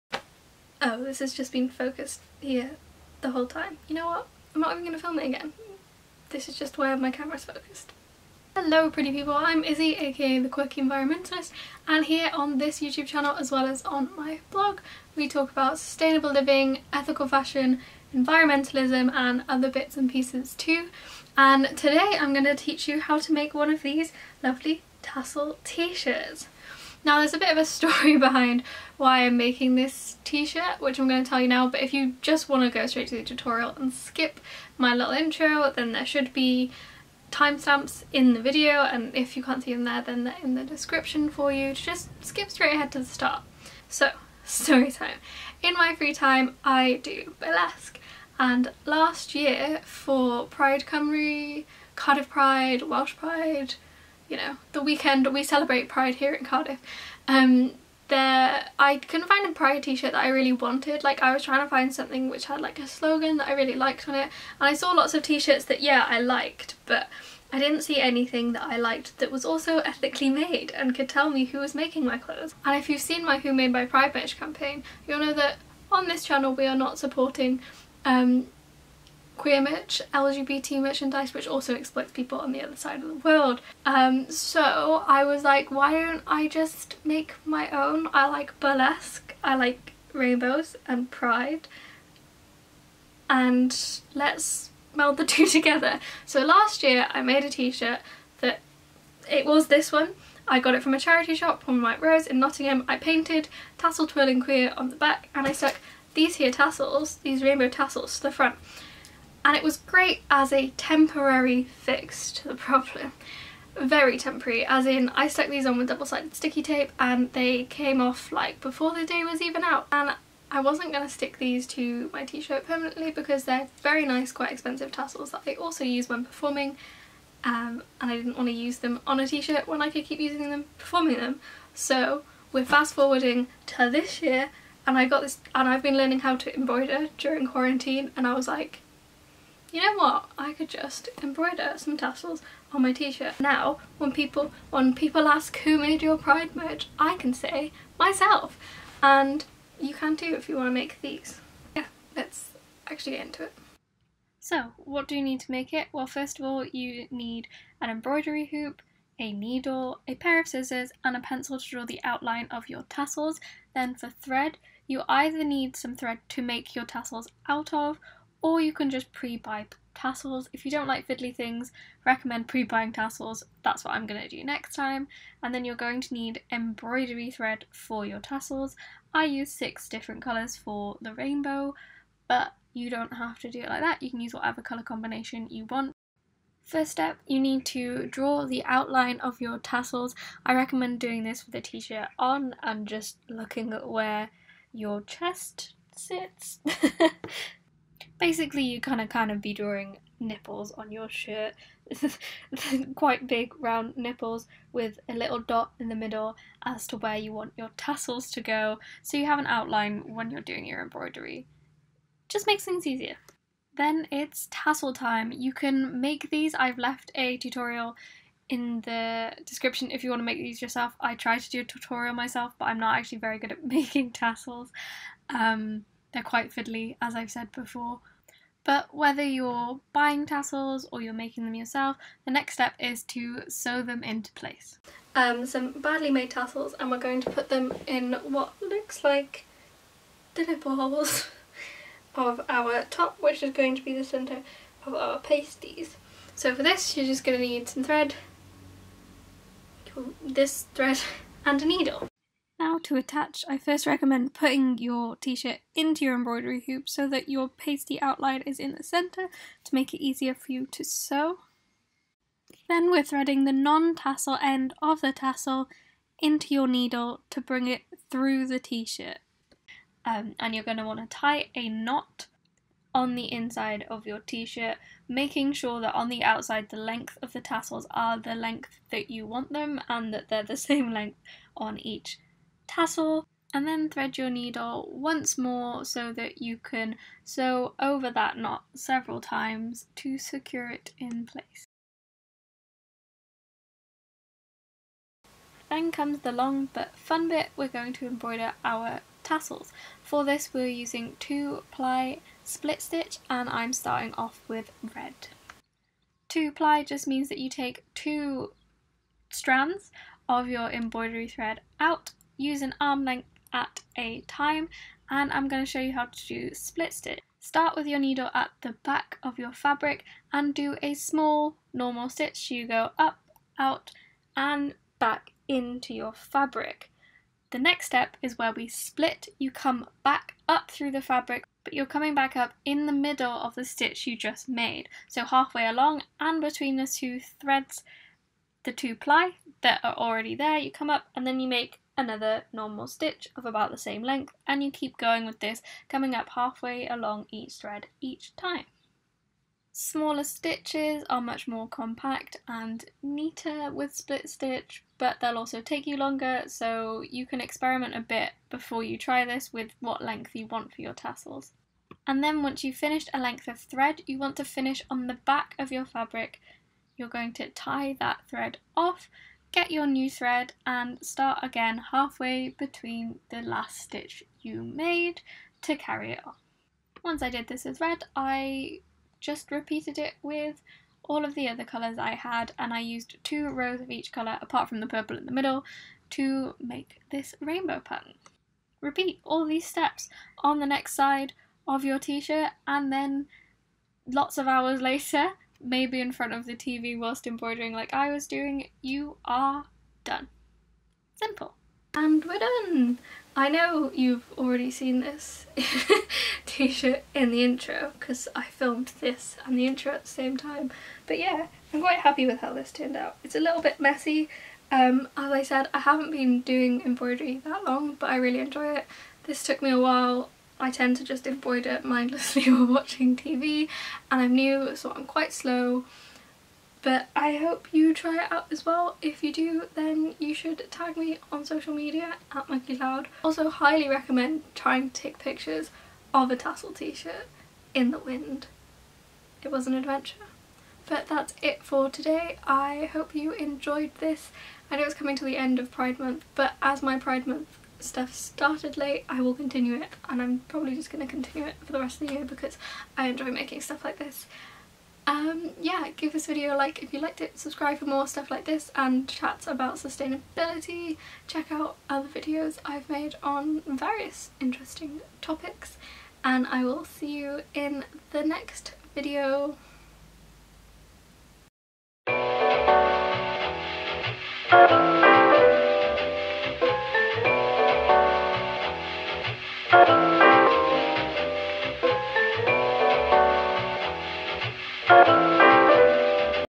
How to Make a Rainbow Tassel Tee for Pride
Who made my pride Merch? Me!

Today I'm showing you how to make this very funky pride t shirt ft. some embroidered rainbow tassels. Fun stuff. Happy pride :)